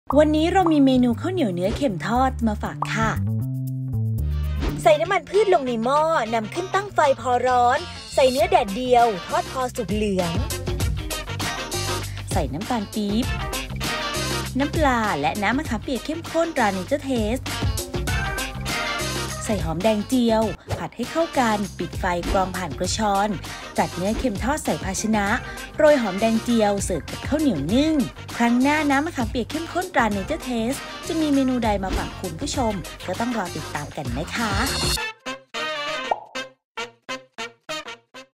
ข้าวเหนียวเนื้อเค็มทอด สูตรเด็ด ทำง่าย สไตล์ อ.ยิ่งศักดิ์
ข้าวเหนียวเนื้อเค็มทอด
salted sun-dried beef with sticky rice

ผลิตภัณฑ์ น้ำมะขามเปียกเข้มข้น ตราเนเจอร์เทส

เนื้อแดดเดียวหั่นเป็นเส้น   300 กรัม
น้ำมะขามเปียกเข้มข้น ตราเนเจอร์เทส 30 มิลลิลิตร
หอมแดงเจียว    50 กรัม
หอมแดงเจียว สำหรับโรยหน้า
ข้าวเหนียวนึ่ง สำหรับรับประทานคู่

1.ใส่น้ำมันพืชลงในหม้อ นำขึ้นตั้งไฟพอร้อน ใส่เนื้อแดดเดียว ทอดพอสุกเหลือง
2.  ใส่น้ำตาลปี๊บ น้ำปลา และ น้ำมะขามเปียกเข้มข้น ตราเนเจอร์เทส  ผัดจนน้ำตาลปี๊บละลาย  ใส่หอมแดงเจียว ผัดให้เข้ากัน  ปิดไฟ กรองผ่านกระชอน
3.จัดเนื้อเค็มทอดใส่ภาชนะ โรยหอมแดงเจียว เสิร์ฟกับข้าวเหนียวนึ่ง

▲Line @yingsakfood